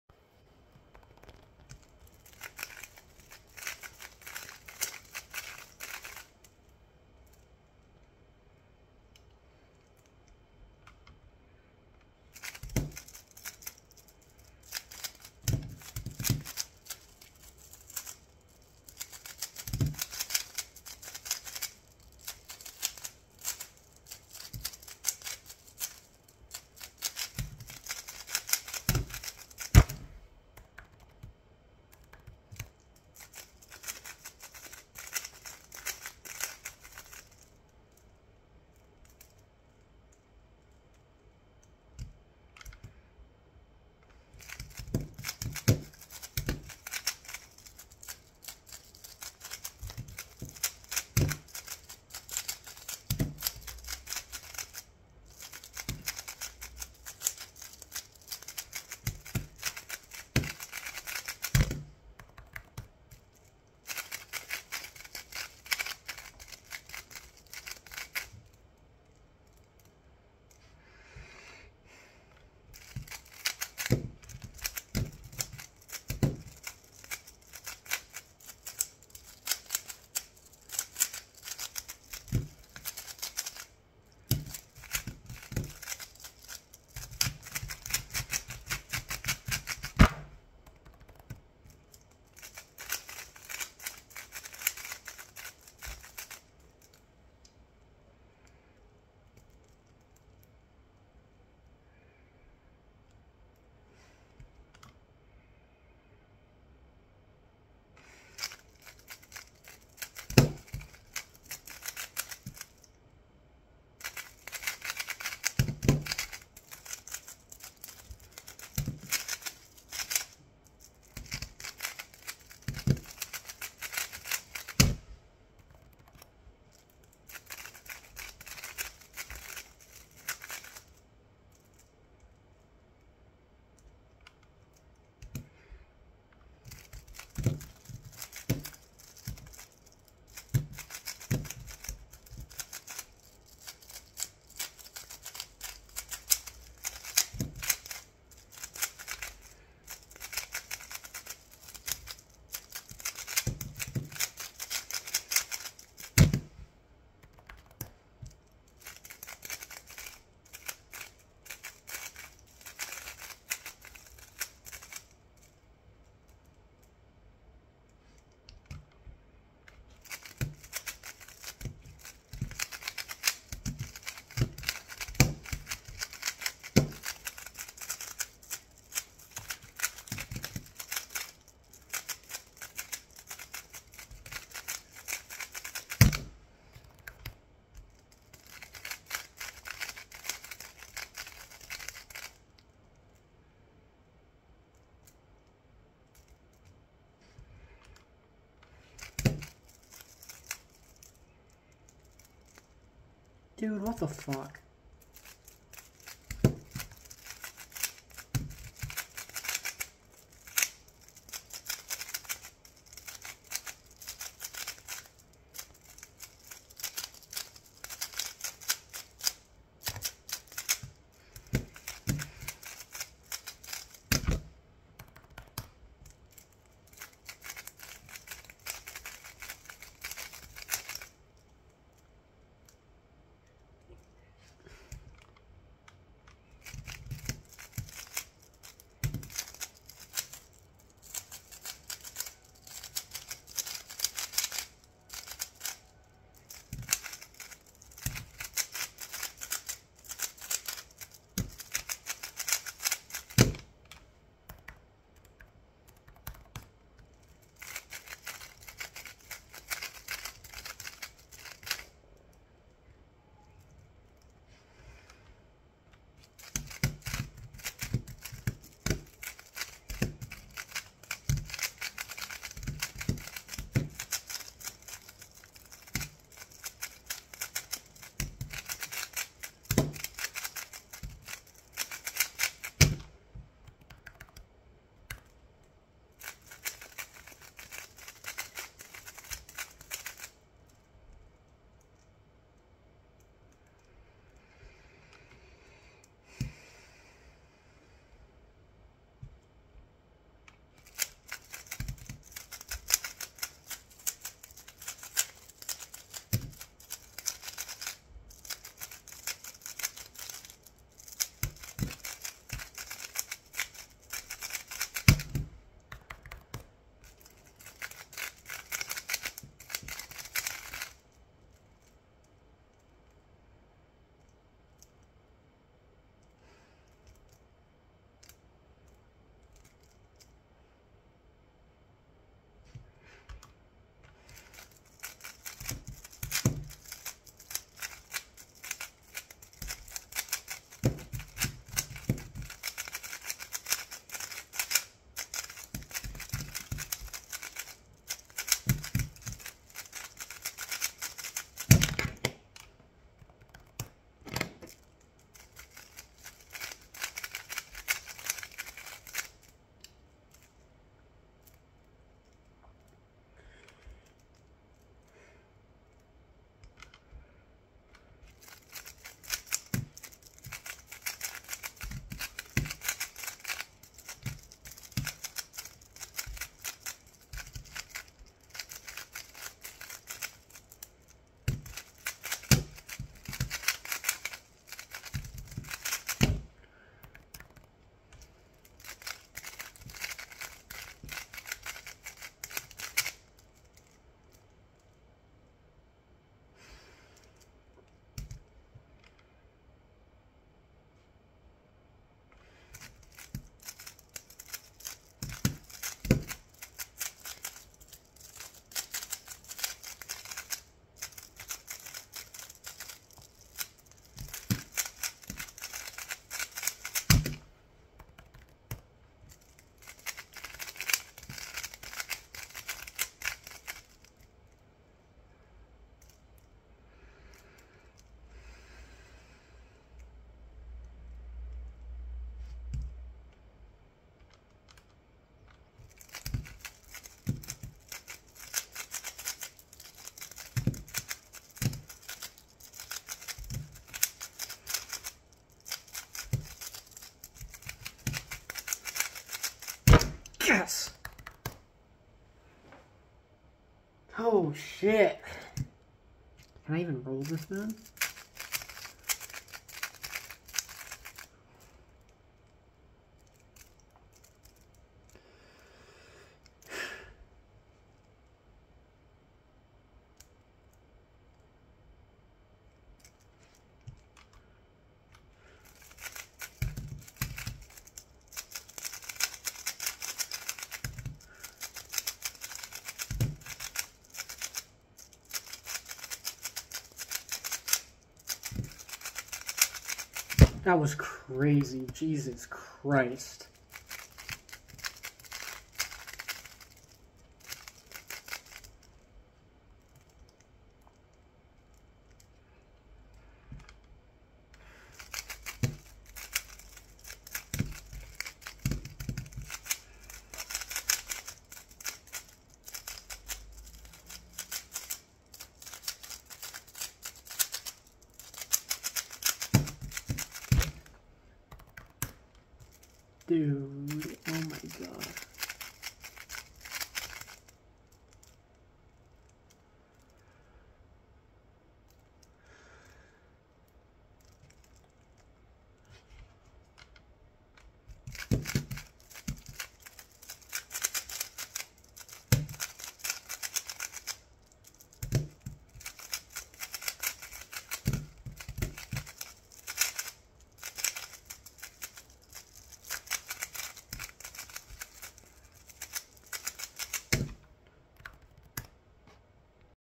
16.64 One Handed ao12
I've been having some fun with OH recently. Got my first sub 17 ao12 (I might've gotten one in 2022 but idr)

I decided to record and got another one!

Average: 17.05 (σ = 1.55)
Mean: 17.39

タイム一覧:
1. 17.72   D2 L2 F' L B' U B' L' U2 D R2 B2 R2 B2 R2 U2 L2 U' R2 F
2. 17.45   D2 F' B2 R2 D2 F2 U' L2 B2 U F2 L2 U2 B L2 F2 L' R' B' D'
3. 17.60   U2 R' U2 F2 U2 F2 R F2 L' F2 D2 L2 D U' B' L D U' L U' B2
4. 16.77   U F' L2 U2 F2 D' L2 U' F2 R2 D2 R2 U2 L D2 B R' U2 B U' R
5. 19.76   B2 L2 F L2 F L2 D2 R2 D2 B U' R2 B R B' F L U' R F
6. 16.29   D B' U D2 F2 R D' L2 B L2 F U2 R2 F' U2 F' B2 U2 D' R
7. 25.60   D' L2 U' F2 L2 U L2 F2 D2 F2 U' R' B D2 R' F U' B R B U2
8. 17.15   B F' D2 L2 F D2 F2 U2 R2 F' L2 R' U' L2 F2 U R B2 U' R
9. 16.85   R2 B2 D' F2 R2 U F2 D' U2 B2 R B' D L R F L D' R2 U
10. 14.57   R2 F U2 F' R2 F' D2 U2 F2 U2 F2 R' D' F R B D U' B2 R B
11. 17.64   U' B U L B' R U B2 D2 B D2 L2 B D2 B2 R2 F R2 D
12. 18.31   U' R L' F' R B2 R L2 B2 L2 B2 D' B2 D R2 D2 B2 L2 B' D2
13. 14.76   B' U B' D2 R2 L2 B D' L F2 R2 D L2 F2 U F2 U D2 L2 D2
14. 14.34   U B' D2 L2 U2 B2 R' U L2 B2 L2 F' U2 R2 F L2 F' R2 F U2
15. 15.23   D' U' B2 U L2 B2 L2 D U2 F2 L' D R2 U2 R' B R2 U2 B L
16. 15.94   R L U R' B' R' L2 F R2 U2 R' B2 R' U2 L' B2 R D2 L' F2 D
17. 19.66   R' U2 F2 R F L D' U2 F' B2 R2 D2 B' R2 L2 B U2 L2 D'